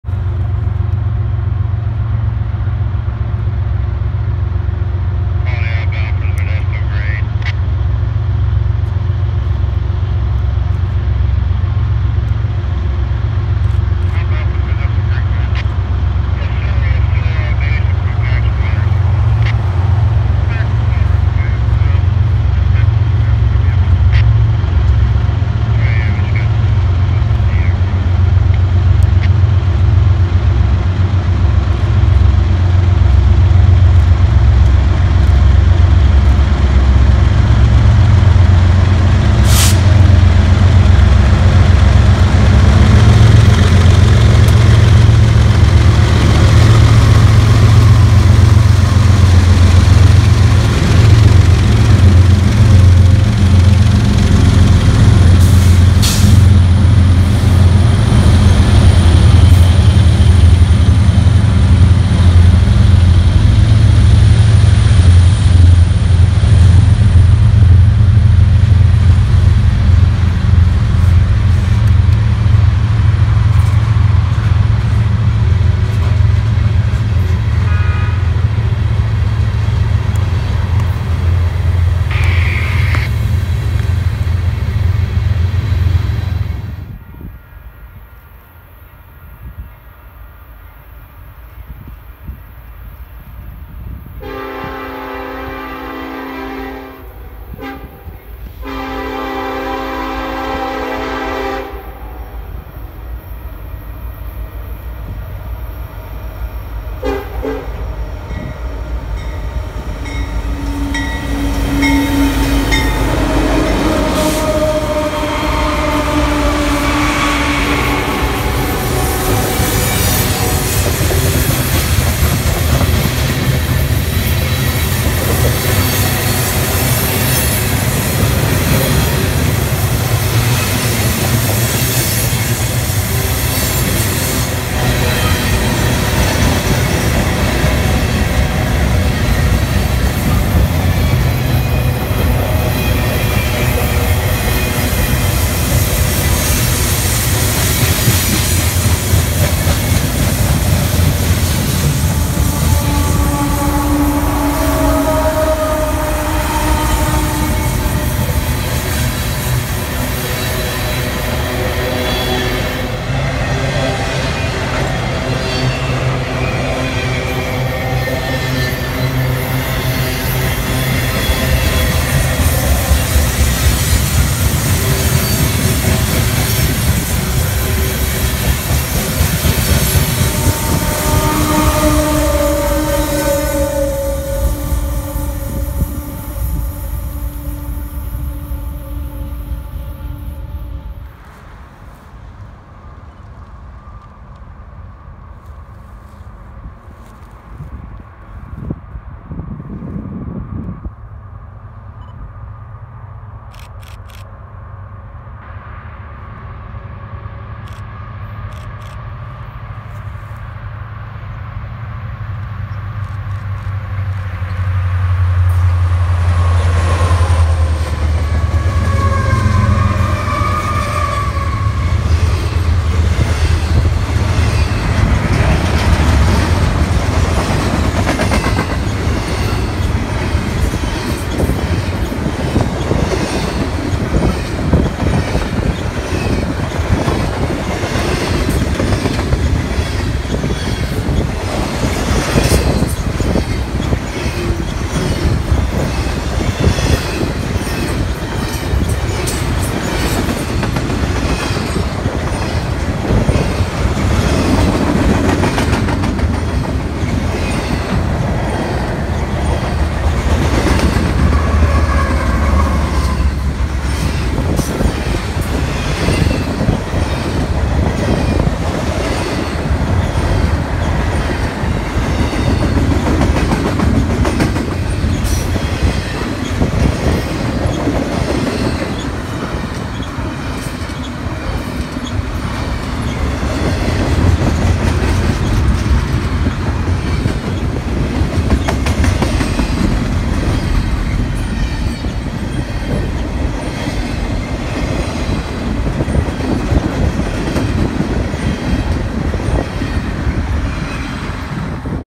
Weekend Highlights featuring 2 heritage leaders!
Weekend filled with 2 heritage leaders! A surprising weekend has came with unexpected heads ups! I saw UP1988 for the second time in a week and UP1989 leading on camera for the first time. Hope you guys enjoy!

Filmed on February 27th, 29th and March 1st, 2020.